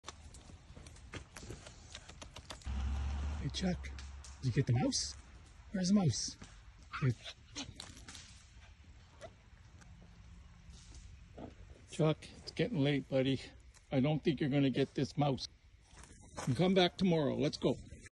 Dawn till dusk 😂Maltese yorkie Chuck works long hours🌠
Morkie Chuck never gives up. He takes his job very seriously and puts in the hours necessary to accomplish his task….aka getting those rats 🤣.
No 9 to 5 hours around this farm 😜🐭🐀